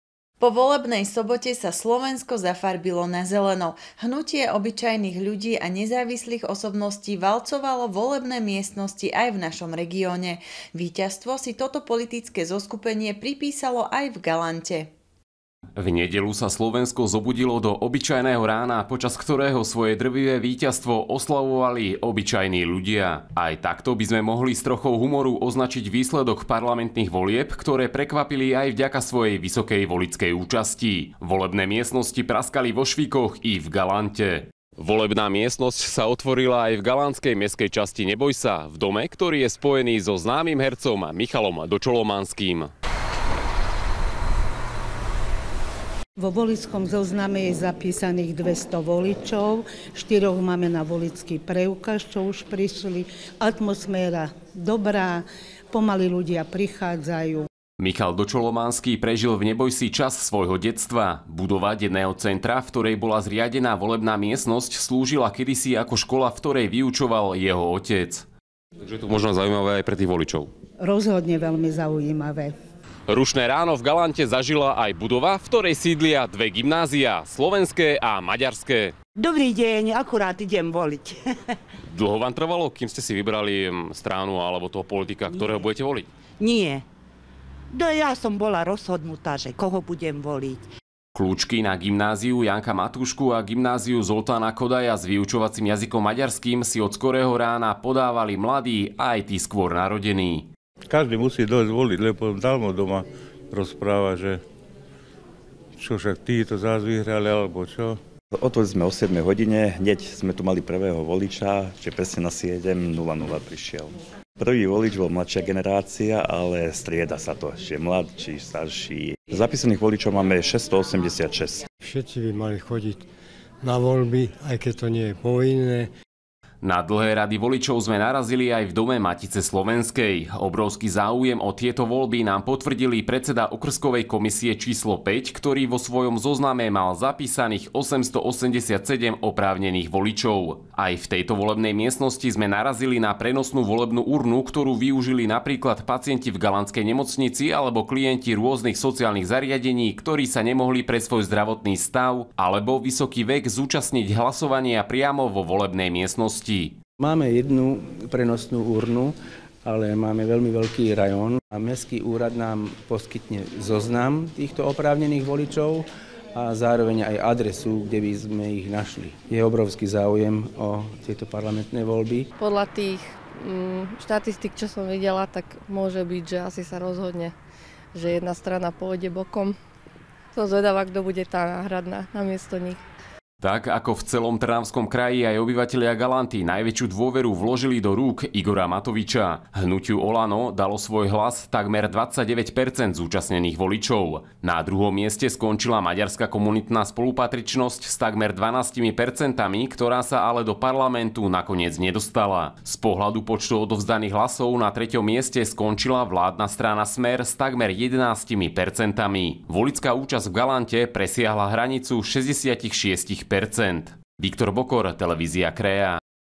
AKO VOLILA GALANTA?
Po volebnej sobote sa Slovensko zafarbilo na zeleno. Hnutie obyčajných ľudí a nezávislých osobností valcovalo volebné miestnosti aj v našom regióne. Víťazstvo si toto politické zoskupenie pripísalo aj v Galante.